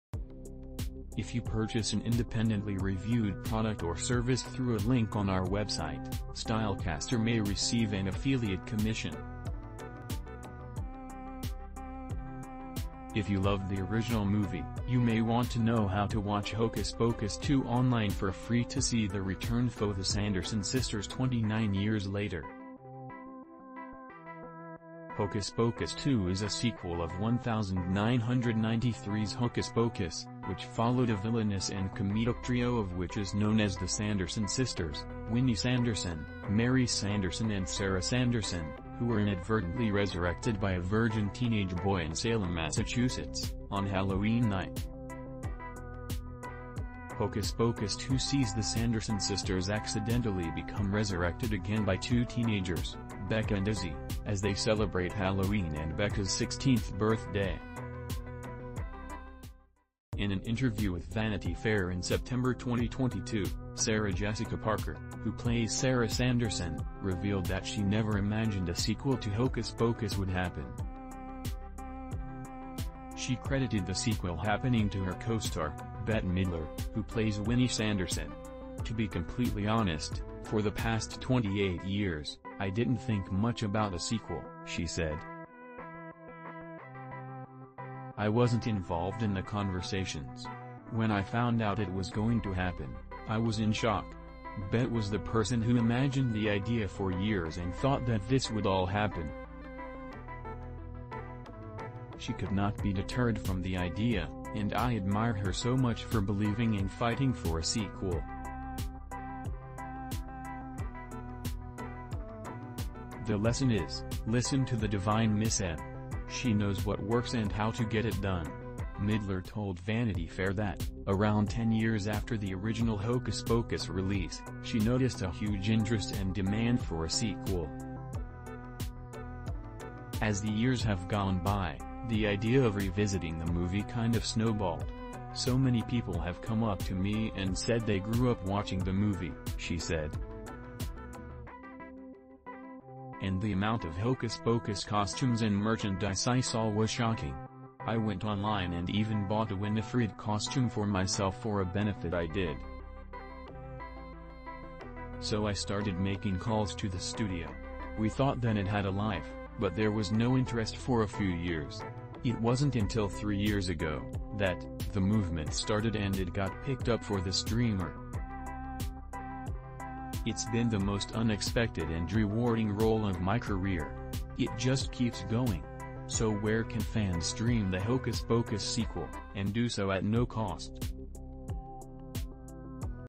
Here’s How to Watch ‘Hocus Pocus 2’ For Free to See the Return of the
If you loved the original movie, you may want to know how to watch Hocus Pocus 2 online for free to see the return fo the Sanderson sisters 29 years later. Hocus Pocus 2 is a sequel of 1993’s Hocus Pocus, which followed a villainous and comedic trio of witches known as the Sanderson sisters—Winnie Sanderson, Mary Sanderson and Sarah Sanderson—who are inadvertently resurrected by a virgin teenage boy in Salem, Massachusetts, on Halloween night. Hocus Pocus 2 sees the Sanderson sisters accidentally become resurrected again by two teenagers, Becca and Izzy, as they celebrate Halloween and Becca’s 16th birthday. In an interview with Vanity Fair in September 2022, Sarah Jessica Parker, who plays Sarah Sanderson, revealed that she never imagined a sequel to Hocus Pocus would happen. She credited the sequel happening to her co-star, Bette Midler, who plays Winnie Sanderson. “To be completely honest, for the past 28 years, I didn’t think much about a sequel,” she said. “I wasn’t involved in the conversations. When I found out it was going to happen, I was in shock. Bette was the person who imagined the idea for years and thought that this would all happen. She could not be deterred from the idea, and I admire her so much for believing and fighting for a sequel. The lesson is, listen to the Divine Miss M. She knows what works and how to get it done.”Midler told Vanity Fair that, around 10 years after the original Hocus Pocus‘ release, she noticed a “huge interest” and “demand” for a sequel. “As the years have gone by, the idea of revisiting the movie kind of snowballed. So many people have come up to me and said they grew up watching the movie,” she said. “And the amount of Hocus Pocus costumes and merchandise I saw was shocking. I went online and even bought a Winifred costume for myself for a benefit I did. So I started making calls to the studio. We thought that it had a life, but there was no interest for a few years. It wasn’t until three years ago [that] the movement started and it got picked up for the streamer. It’s been the most unexpected and rewarding role of my career. It just keeps going.”So where can fans stream the Hocus Pocus sequel—and do so at no cost? Read on for how to watch Hocus Pocus 2 online for free to see the return of the Sanderson sisters almost three decades later. Image: Disney+ / Courtesy Everett Collection. When does Hocus Pocus 2 come out? Hocus Pocus 2 premiered on September 30, 2022, at 3:01 a.m. ET/12:01 a.m. PT on Disney Plus. How to watch Hocus Pocus 2 onlineHocus Pocus 2 is available to stream on Disney Plus, which costs $7.99 per month or $79.99 per year (which saves users about $16 from the monthly price.) Disney Plus is also a part of The Disney Bundle, which includes Hulu, Disney Plus and ESPN Plus for $13.99 per month for Hulu with ads and $19.